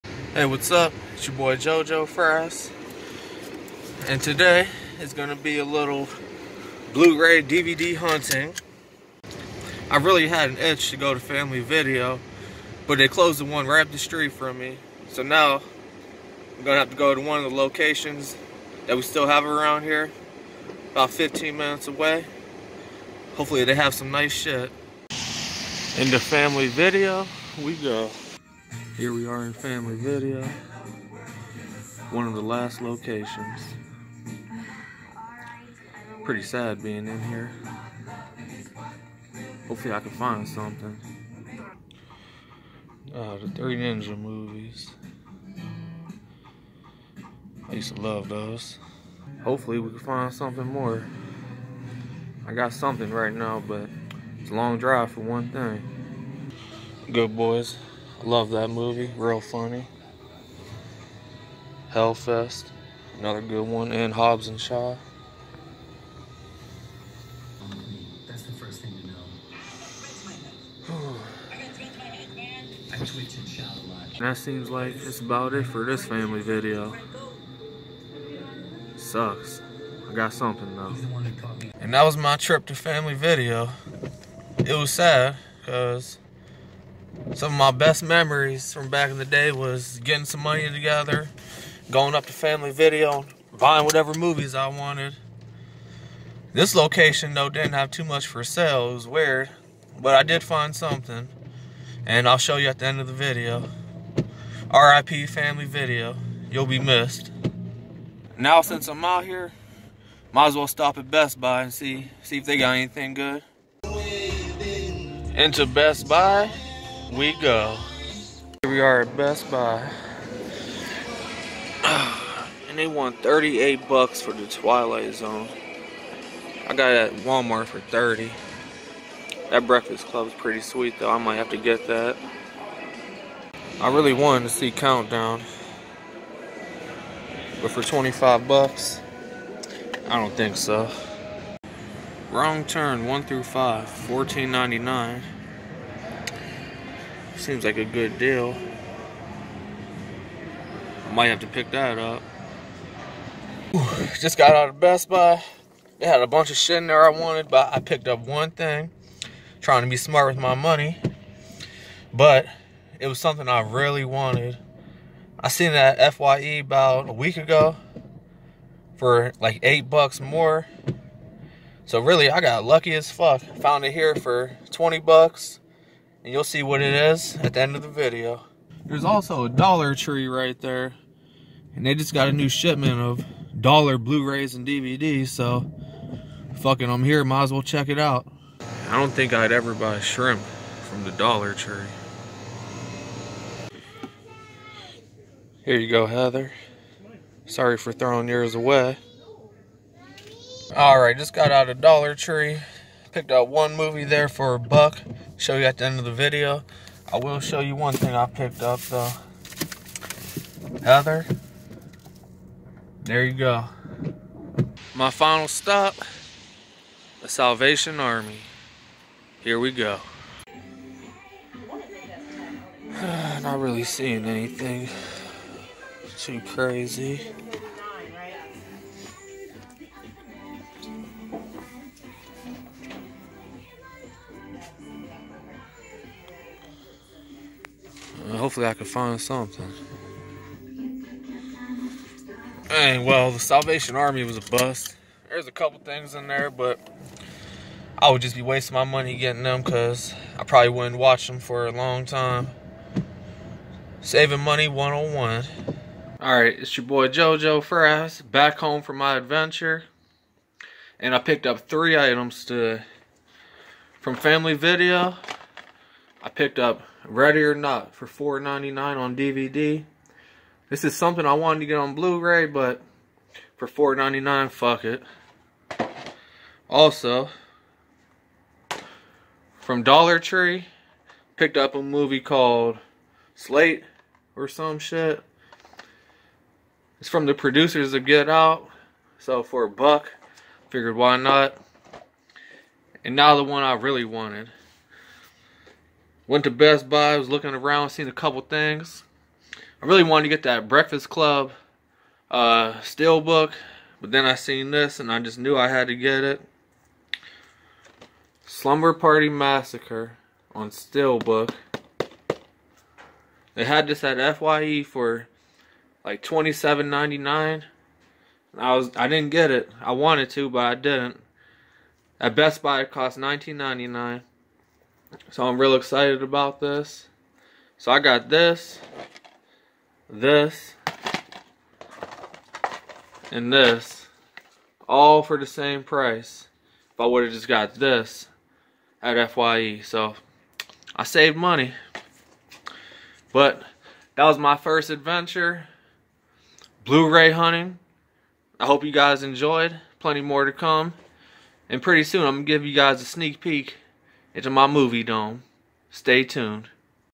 Blu-Ray/DVD Hunting--Family Video,Best Buy, Dollar Tree
Wetmovie1 And Coolduder Inspired Me to start doing vidoes like this. I been a movie collecter my whole like but never recorded my trips. so here is the first one. went to family video,best buy and dollar tree. not the biggest haul but some good titles I picked up . watch and enjoy